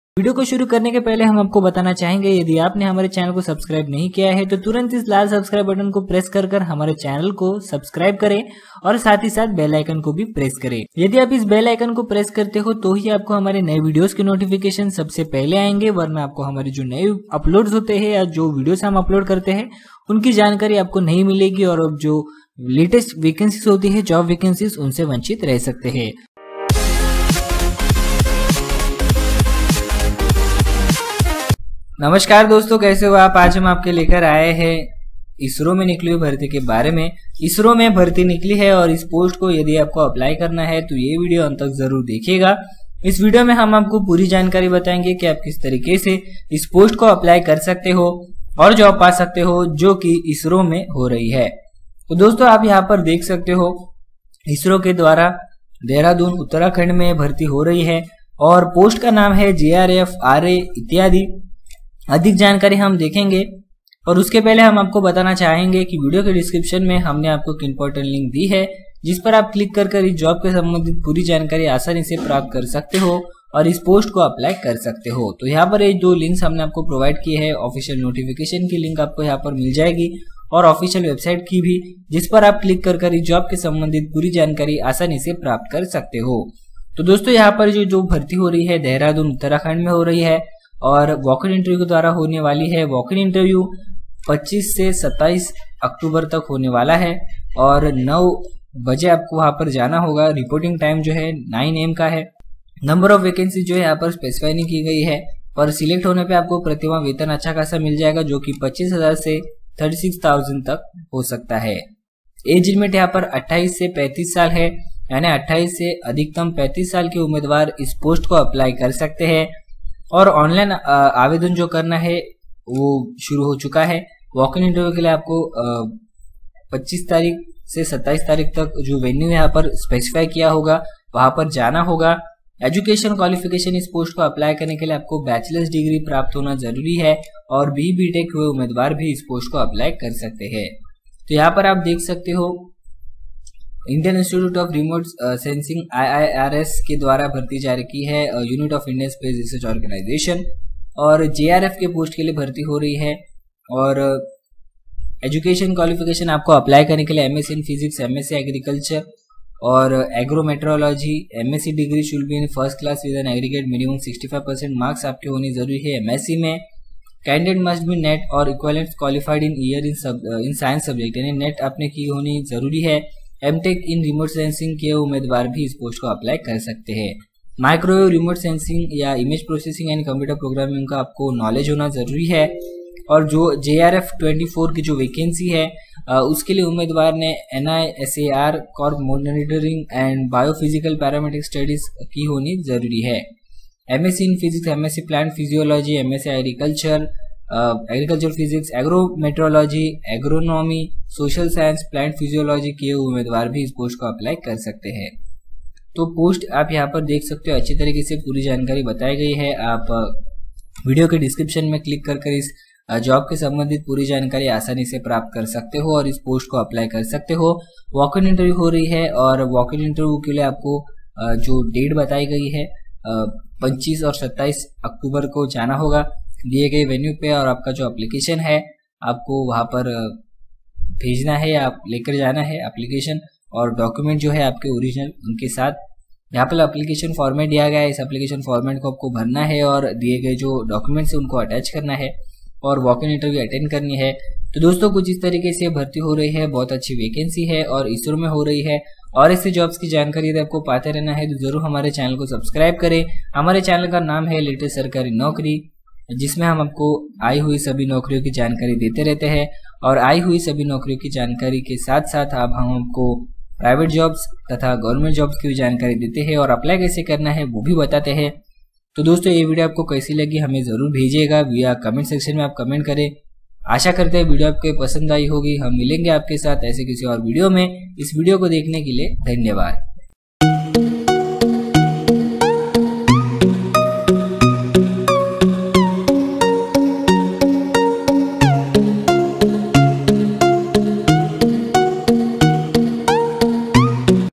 ISRO Recruitment 2018 - Walk-in-Interview - Jobs in October - Apply Now !
Today's job about ISRO – Indian Institute of Remote Sensing (IIRS) Recruitment 2018. The following job is on Regular basis. For more detailed updates kindly visit the link given Above. You can  apply for this job through the link provided